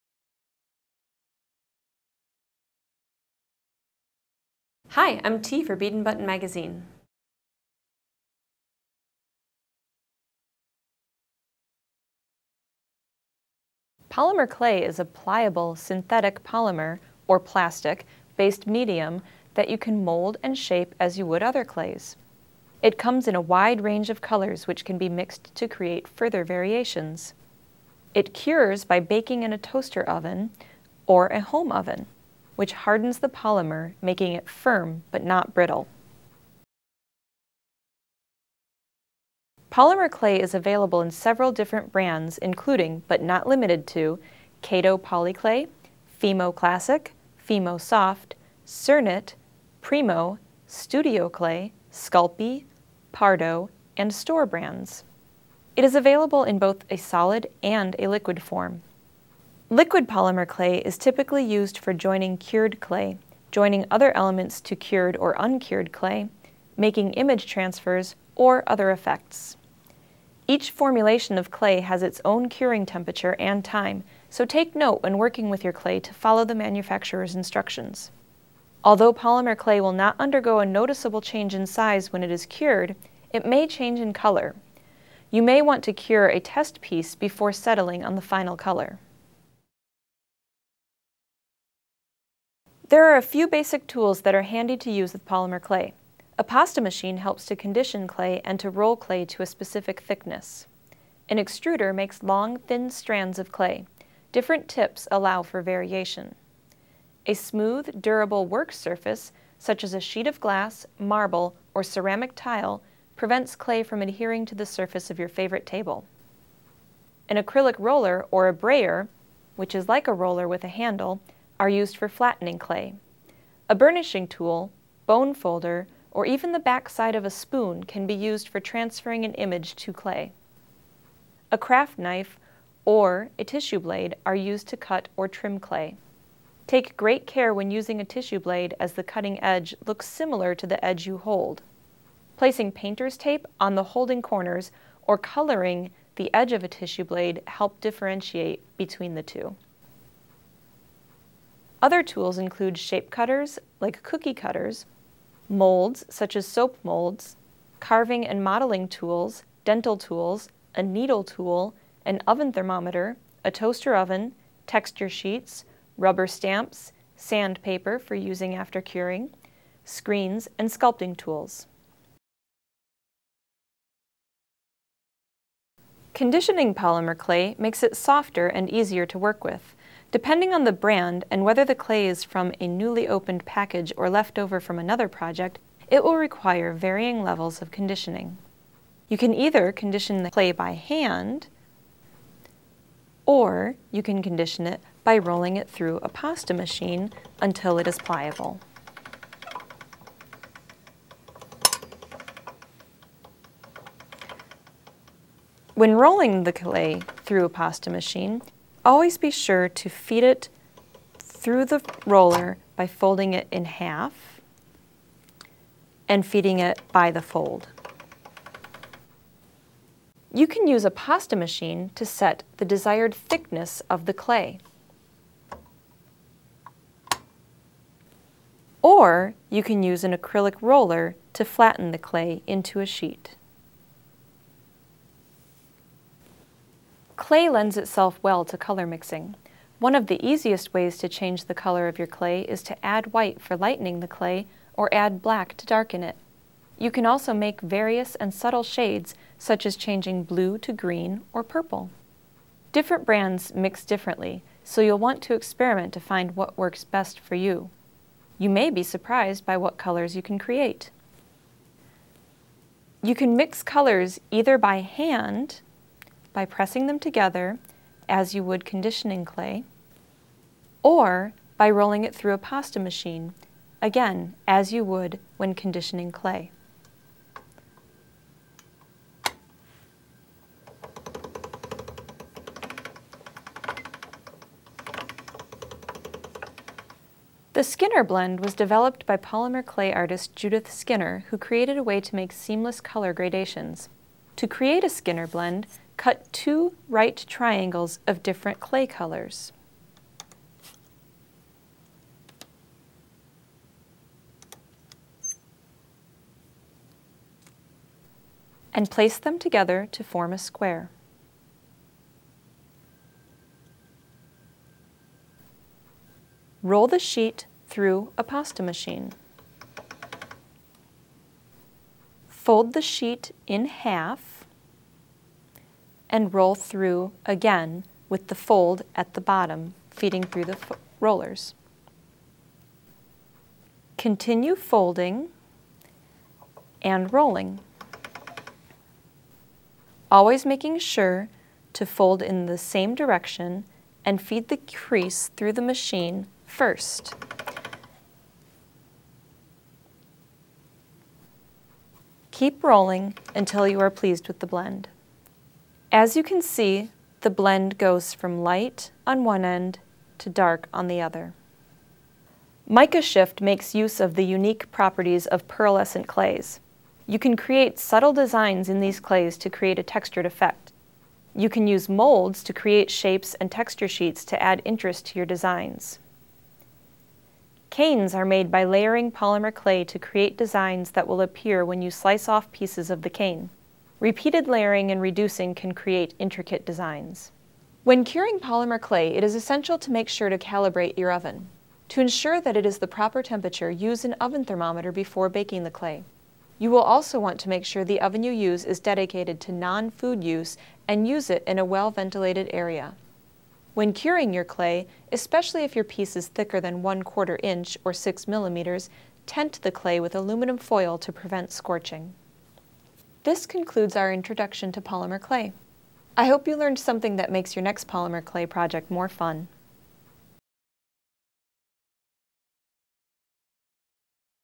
Intro Polymer Clay
An introduction to what polymer clay is, who the major manufacturers are, what benefits it offers to jewelry artists, and an overview of what jewelry artists are doing with it.